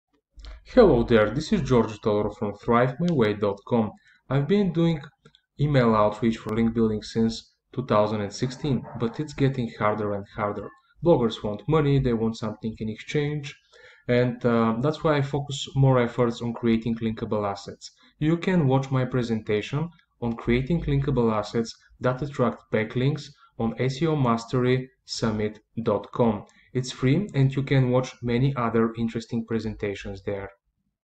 Georgi Todorov Teaser
📢 Announcing another excellent SEO Mastery Summit speaker, Georgi Todorov! seomasterysummit.com

Georgi is a mastermind at creating linkable assets that attract quality backlinks - catch his talk on this topic, along with 40+ other SEO experts.